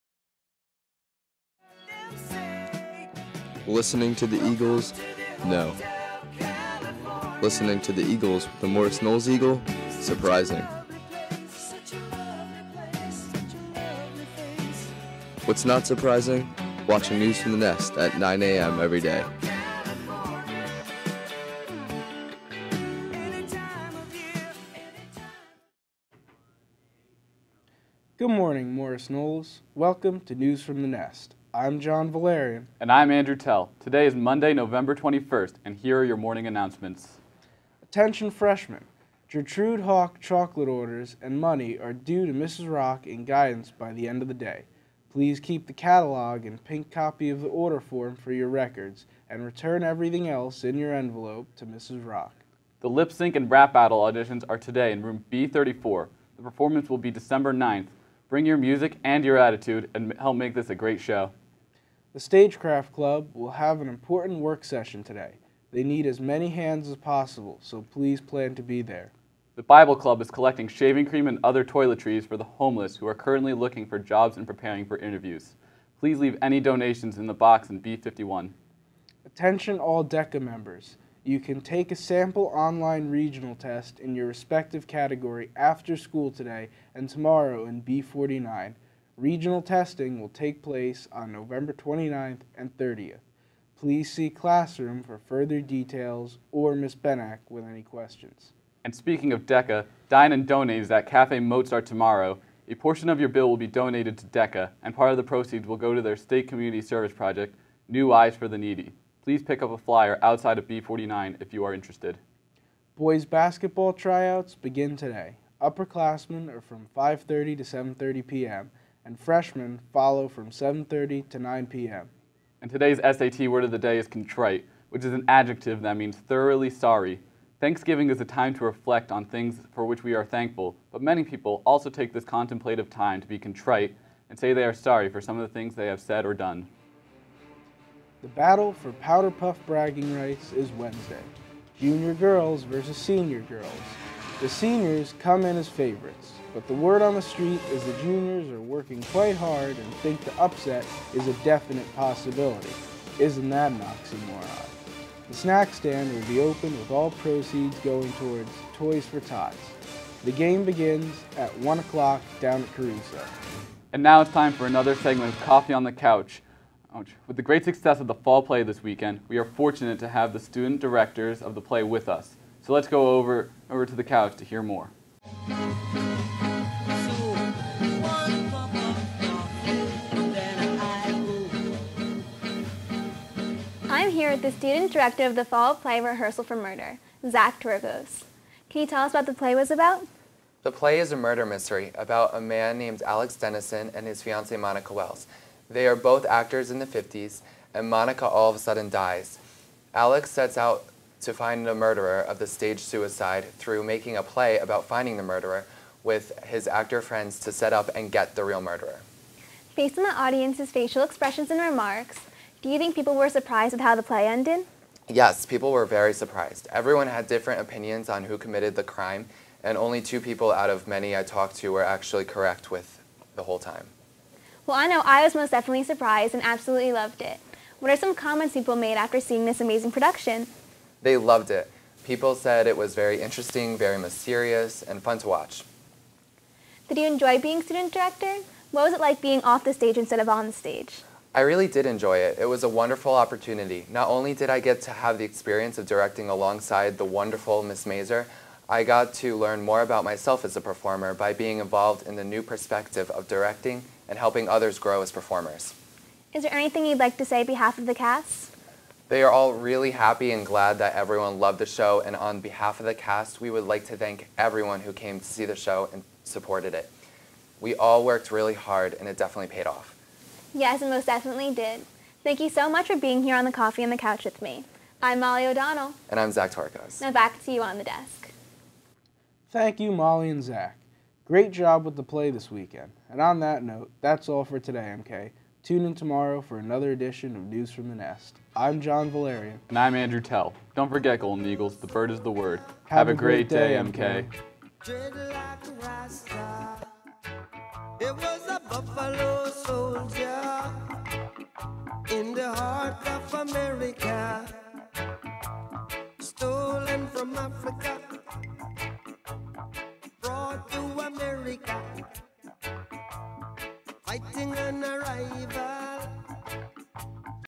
News From The Nest - November 21, 2016
November 21 edition of Morris Knolls' student produced daily news show News From The Nest.  Includes Powder Puff Promo and Coffee On The Couch for the Fall Play